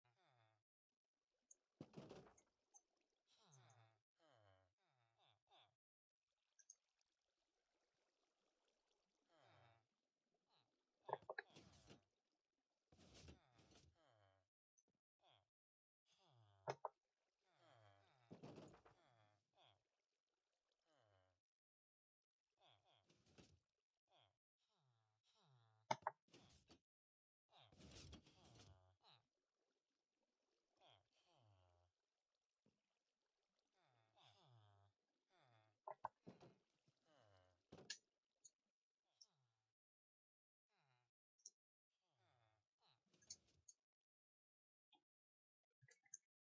Villager vending machine
This was a collaboration with a friend of mine. This came form the ides of my last video on making the villager trading hall a more easy to use villager vending machine hope you enjoy :D